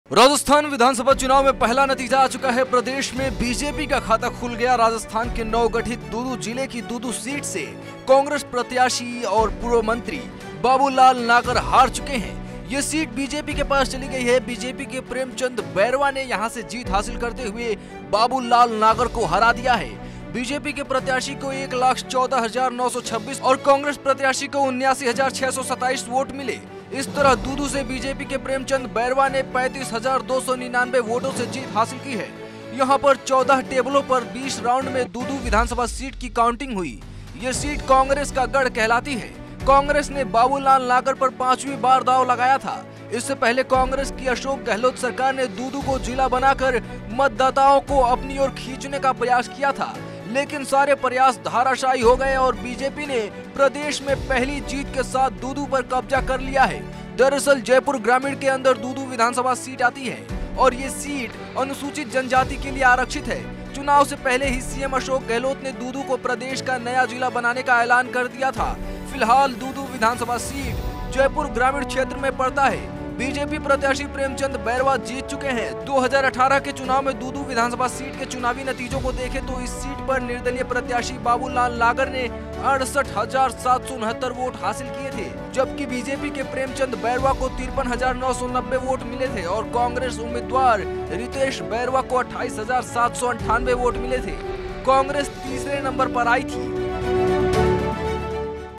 Rajasthan election 2023: कांग्रेस के गढ़ में बीजेपी के प्रेमचंद बैरवा की हुई जीत | bjp
Rajasthan election 2023: कांग्रेस के गढ़ में बीजेपी  के प्रेमचंद बैरवा की हुई जीत ...नवगठित दूदू जिले की दूदू सीट से कांग्रेस प्रत्याशी और पूर्व मंत्री बाबूलाल नागर हार चुके हैं. ये सीट  बीजेपी के  पास चली गई है |